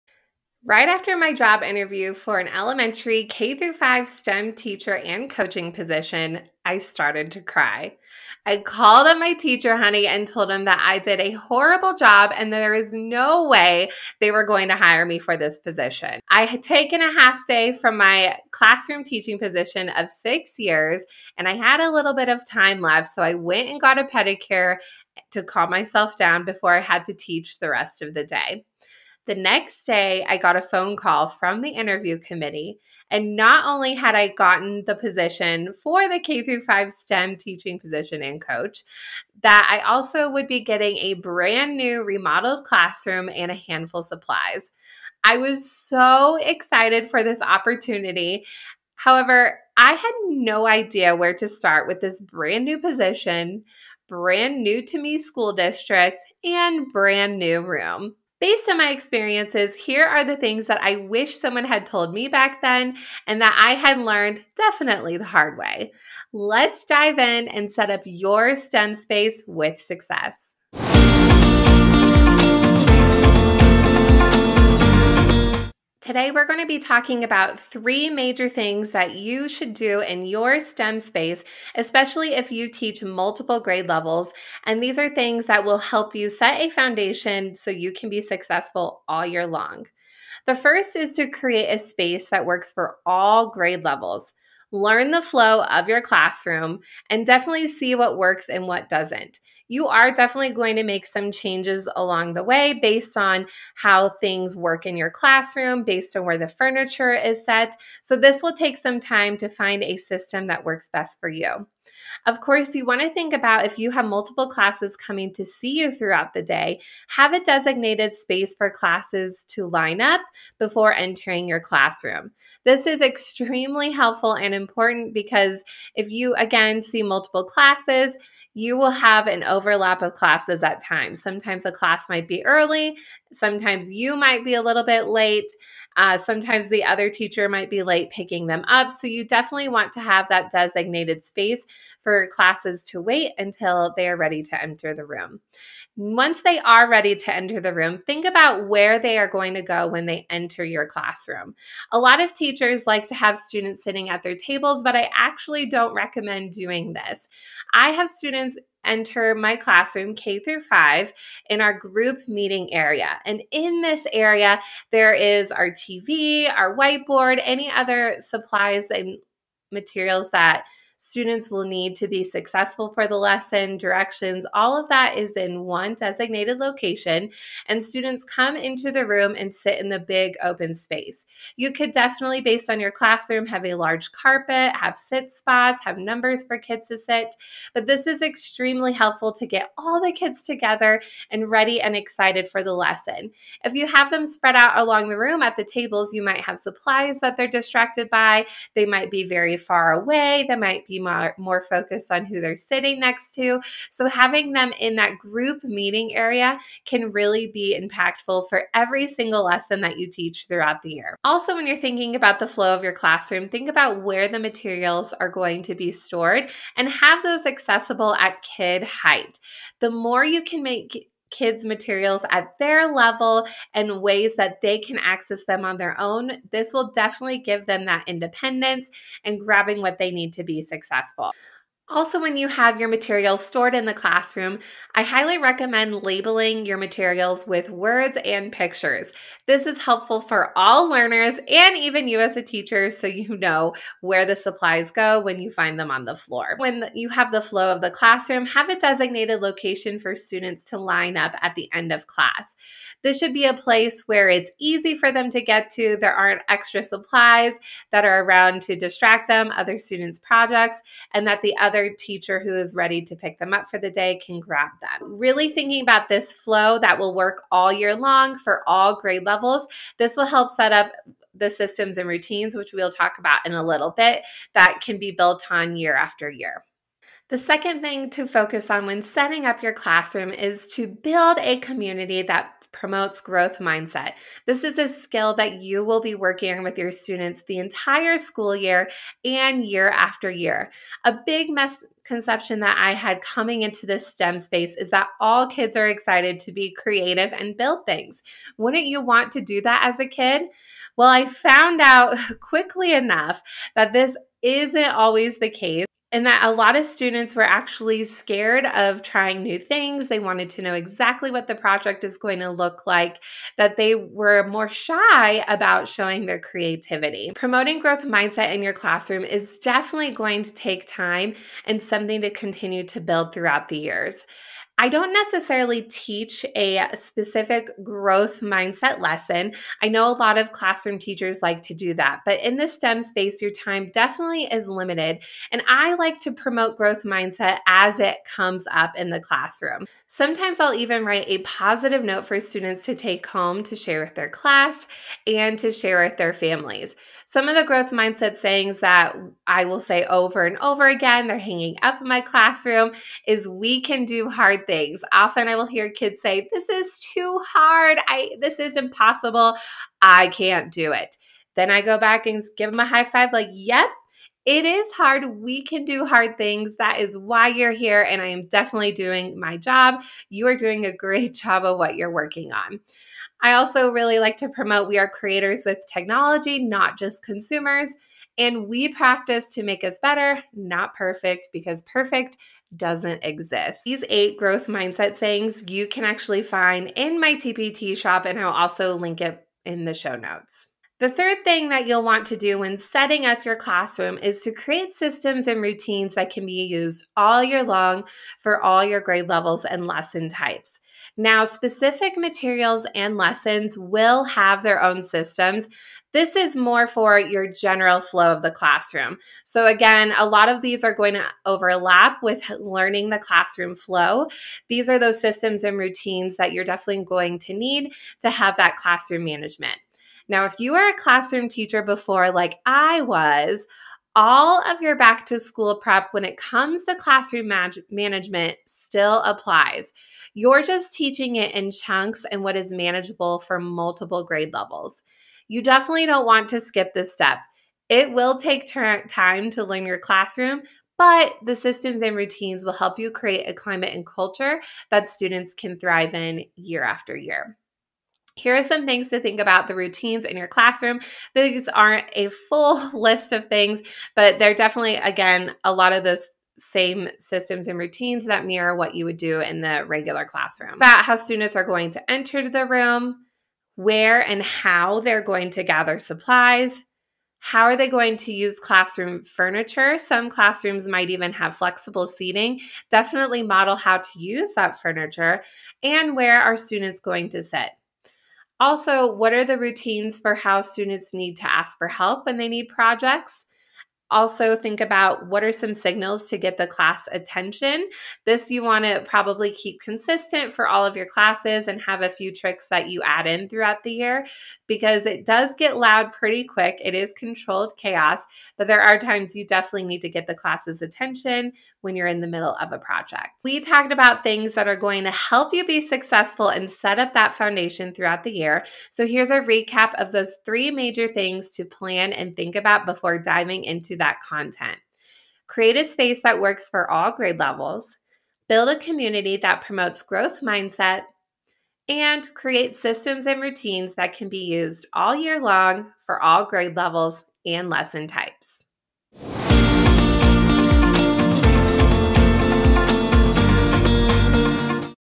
I’m a new STEM Teacher, what should I focus on? Elementary STEM Coach Podcast Ep. 1
After I interviewed and found out I was going to be a K-5 STEM teacher and coach, I not only got the position, but I also would be getting a brand new remodeled classroom and a handful of supplies. I was so excited for this opportunity! However, I had no idea where to start with this brand new position,  brand new-to-me school district, and brand new room.

Based on my experience, this episode will cover some things that I wish someone had told me to do back then (I definitely had learned the hard way!)

Let's dive in and set up your STEM space with success today. We're going to be talking about 3 major things that you should do in your  STEM space. Especially if you teach multiple grade levels, these are things that will help you set a foundation so you can be successful all year long.
This is the video version of The Elementary STEM Coach Podcast.

Head to the show notes for a full transcript of this episode, pictures and all the

Podcast: The Elementary STEM Coach